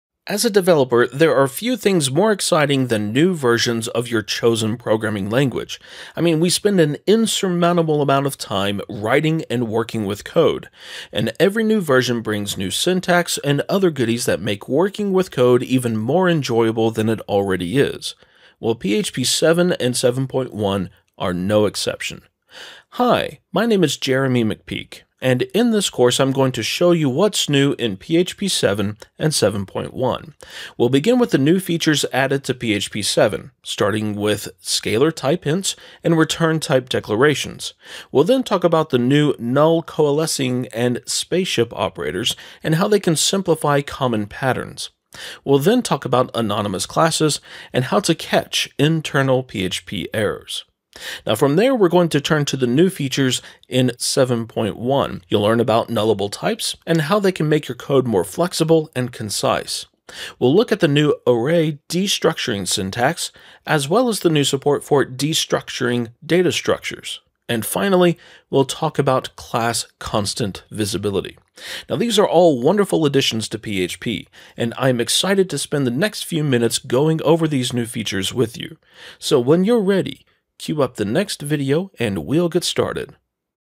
What's New in PHP 7 and 7.1?: Introduction
As a developer, there are few things more exciting than new versions of your chosen language. We spend a lot of time writing and working with code, and every new version brings new syntax and other goodies that make working with code even more enjoyable than it already is. PHP 7 and 7.1 are no exception.

In this course, Jeremy McPeak will take you through each of the new language features in PHP 7 and 7.1. Jeremy will get you up to speed with the latest and greatest for the PHP language, with practical examples of each new feature.